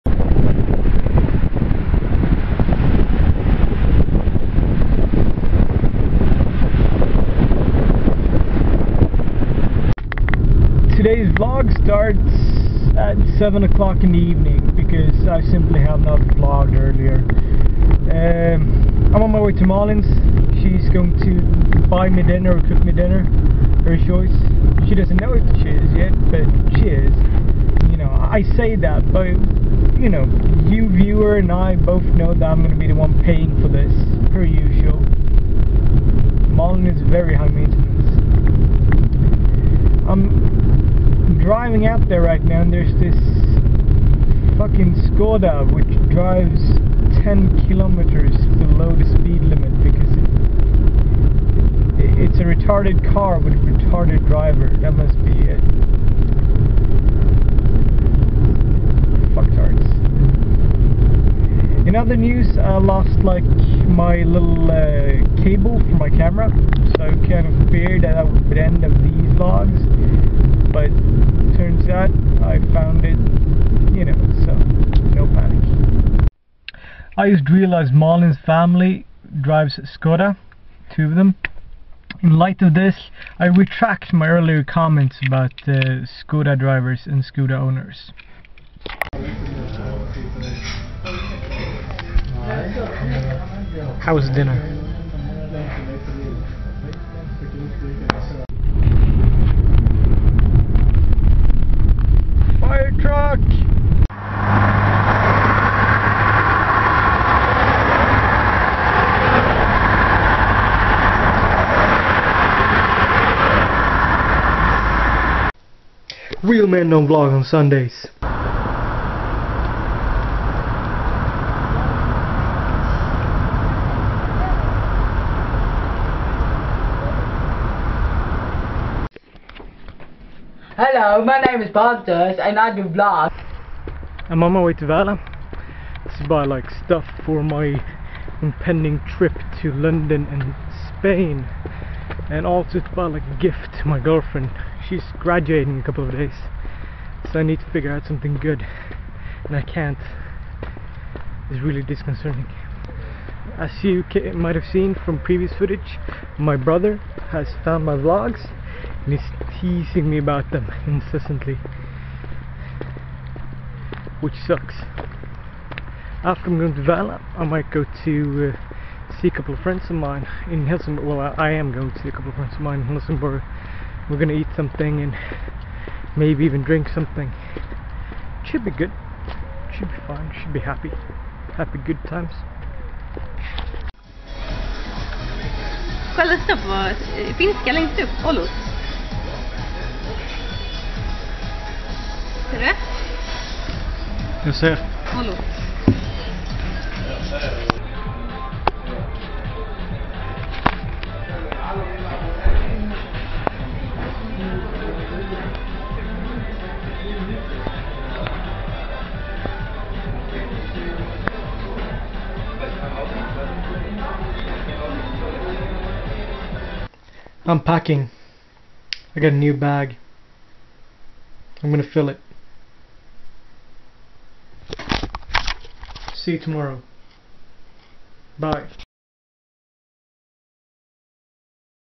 What the hell does "Oullo" mean anyway? (vlog 9, 3rd-5th sept 2011)
In which Pontus spend a lot of time with Malin and is suprisingly halfnaked.